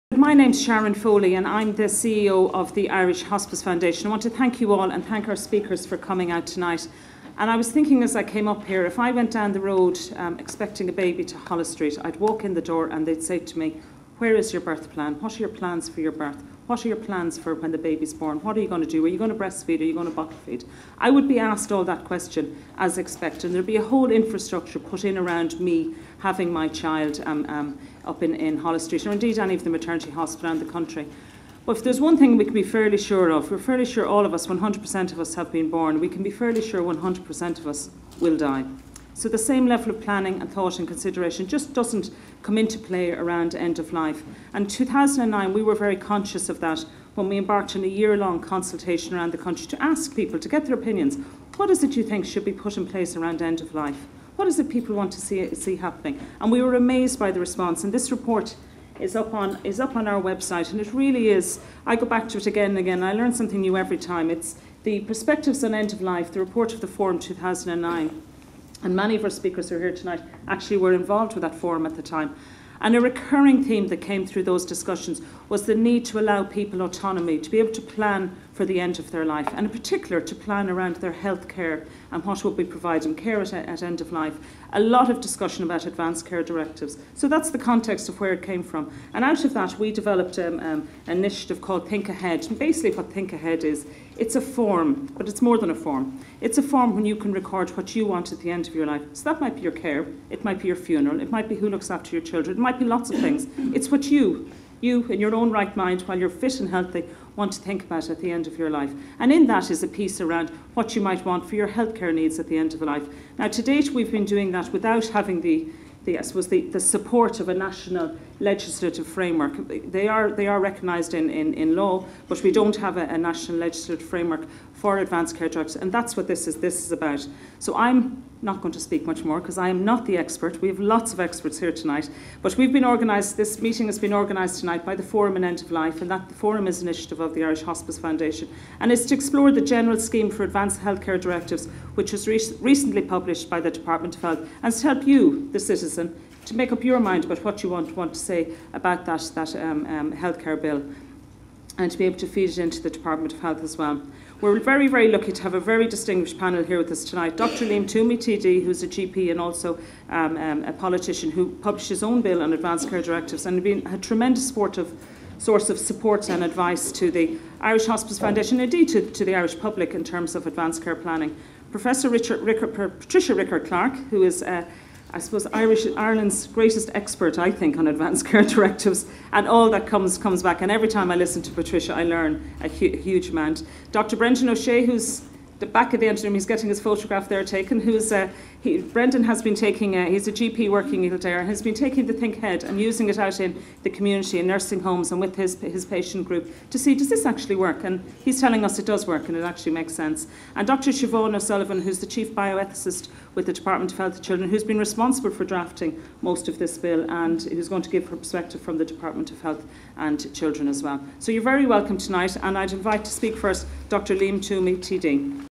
Sharon Foley - Advance Healthcare directives
Sharon Foley, CEO of Irish Hospice Foundation speaking at Public meeting on Advance Healthcare directives Feb 27th 2014